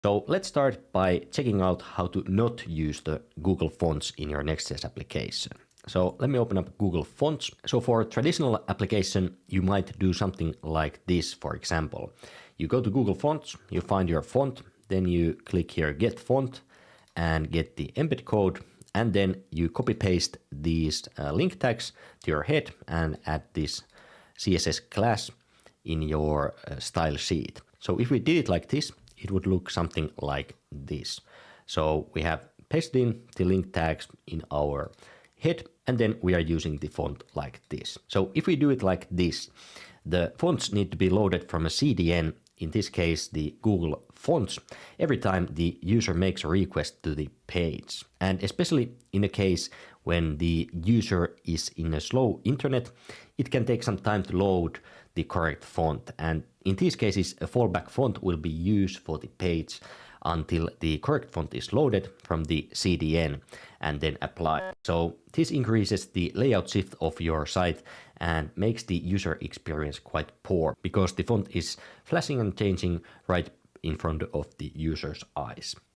How NOT to use Fonts in Next.js
Discover how to enhance user experience in your Next.js application by avoiding common pitfalls with Google Fonts. Learn about the importance of loading fonts efficiently and minimizing layout shifts for smoother page rendering.